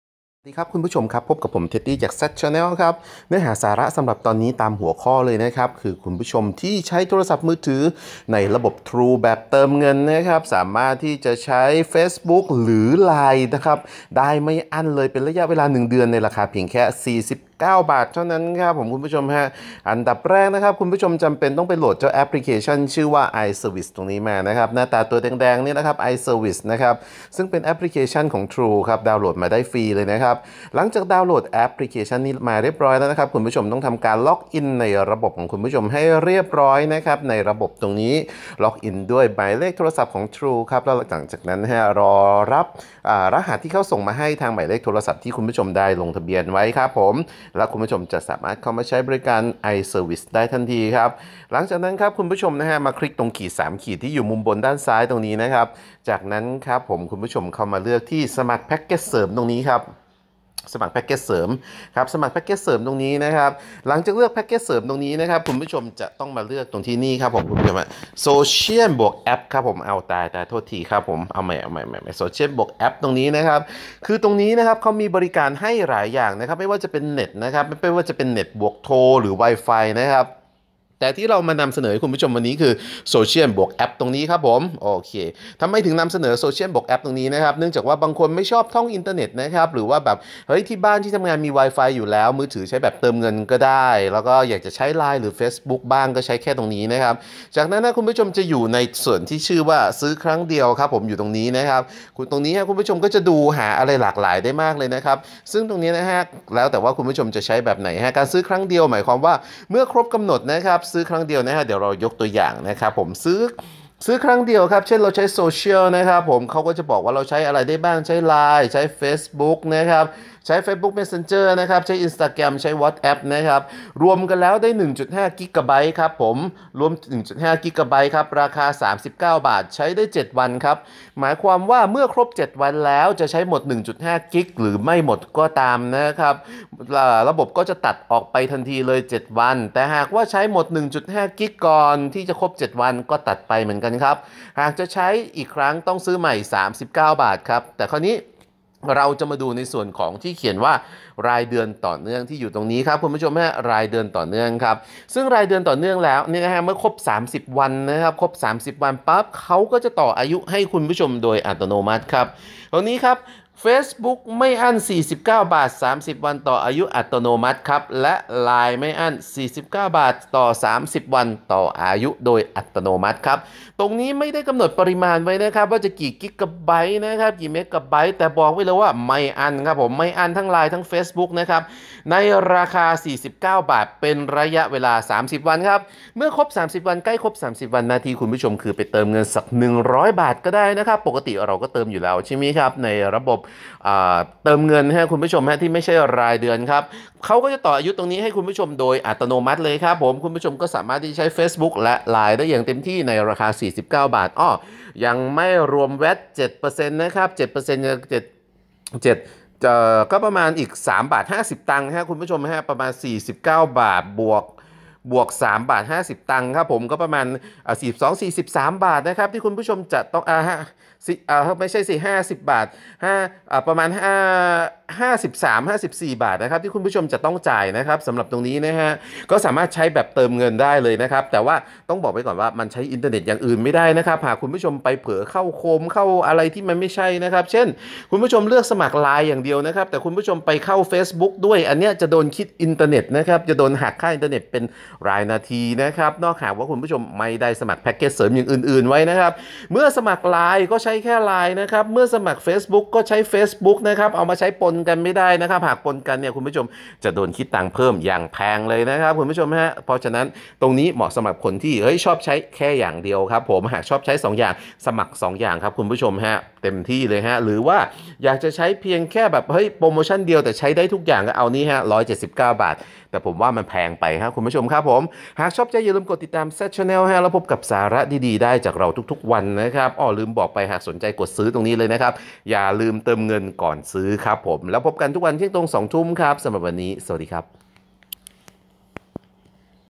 Line / Facebook ไม่อั้น แค่เดือนละ 49 บาท
มือถือระบบเติมเงิน ในระบบ True หากต้องการจะใช้ไลน์ ใช้เฟสบุ๊ค แบบราคาประถูก ประหยัด ก็เรียกใช้บริการเสริมได้ด้วยตัวเอง ในราคาเพียงแค่ 49 บาทต่อเดือน ต่ออายุอัตโนมัติทุก 30 วัน
สาระดีๆกับ Zad Channel ทุกๆวัน